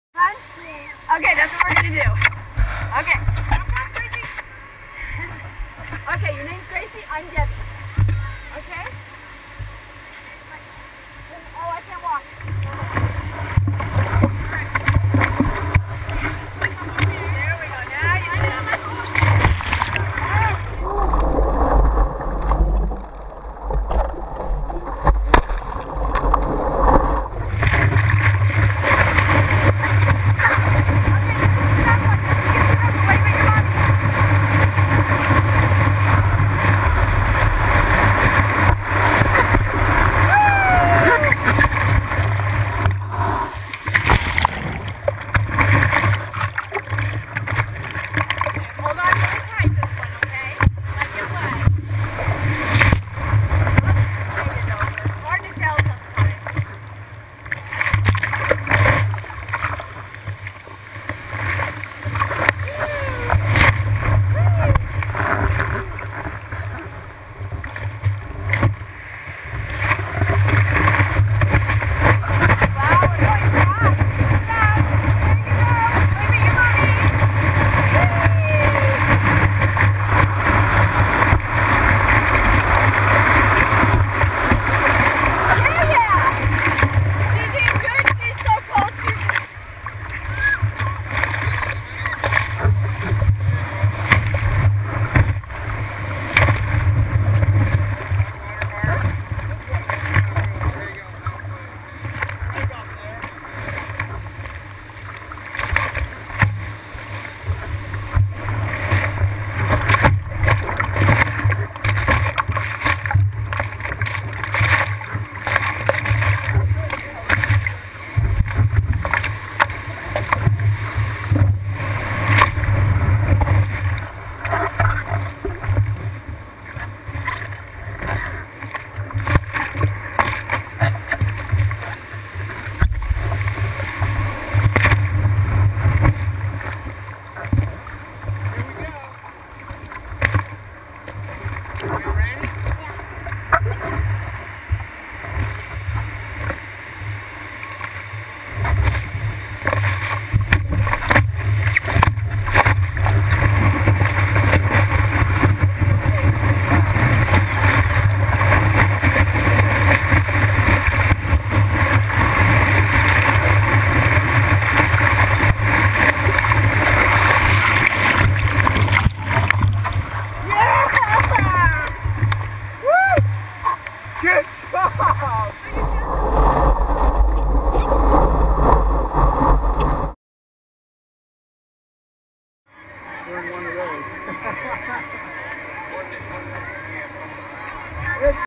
Tandem Surfing with Grace at Oceanside's COOP Soccer/ Mauli-Ola Foundation Booth
Mark & Debbie Tandem Surfing with Grace at the COOP-Swimways Corp-Island Shade Shacks- Mauli-Ola Foundation- Sand Soccer Event held May4th,5th & 6th 2011 at Oceanside Harbor (Watch in HD 1080i)